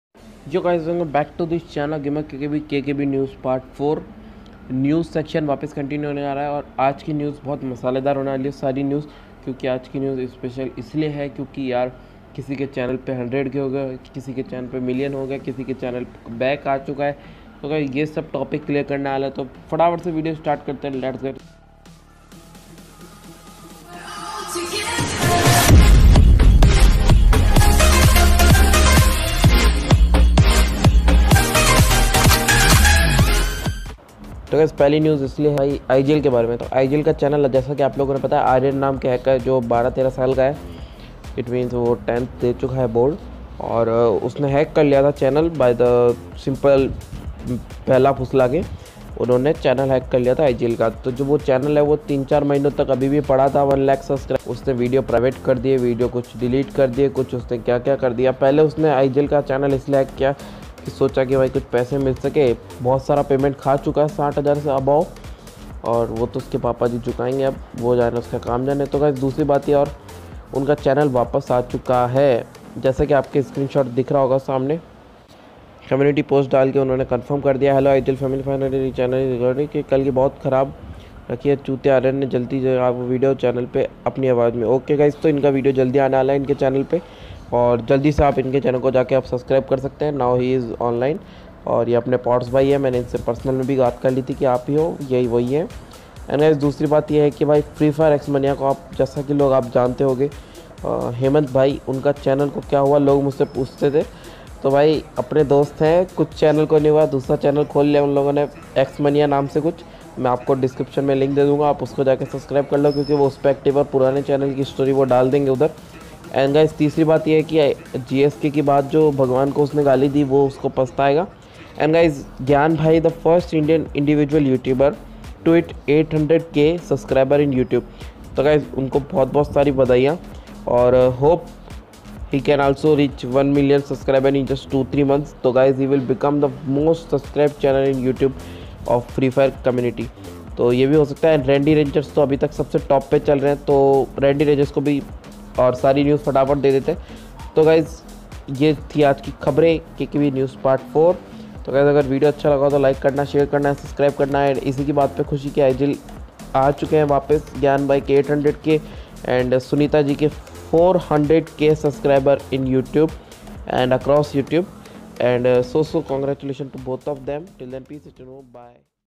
IGL 🤟 CHANNEL IS BACK GOOD NEWS FOR IGL FANS , GYAN GAMING 800K SUBS, X MANIA CHANNEL SUSPENDED???
Hey guys welcome back to kkb news part 4

GYAN GAMING
SONEETA